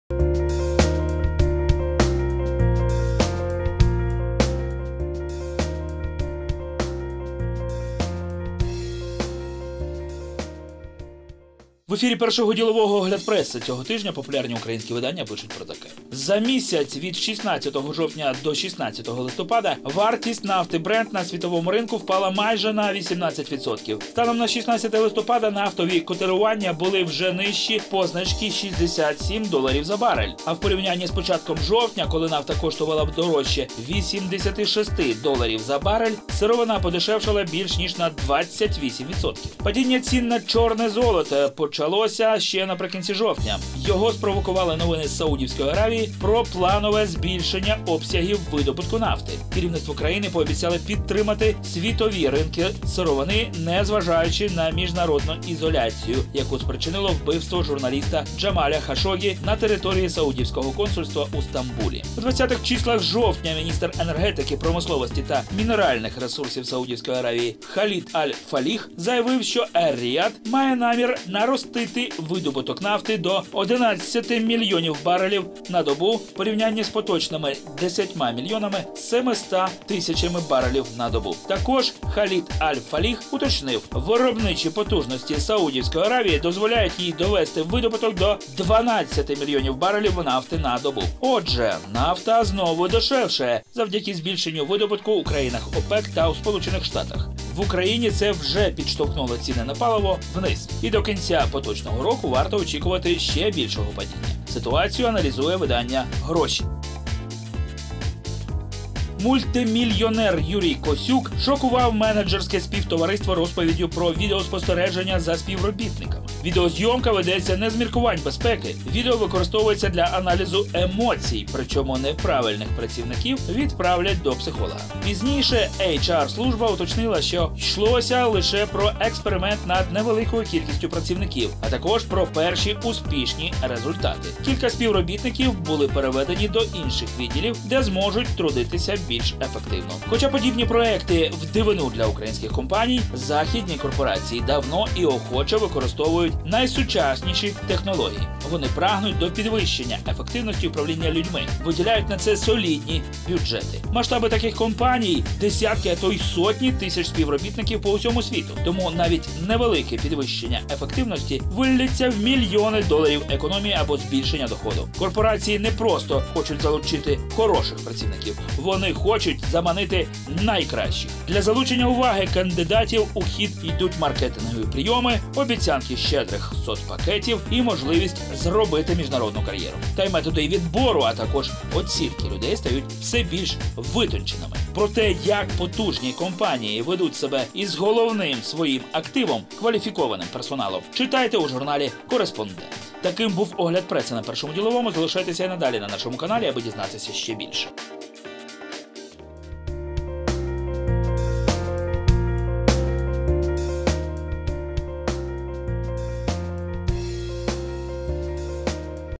Пресс-обзор за 26 ноября
Ситуацию цен на топливо нефть в мире и Украине, анализирует на своих страницах издание "Деньги".
Как мощные компании ведут себя с квалифицированным персоналом, читайте в журнале "Корреспондент".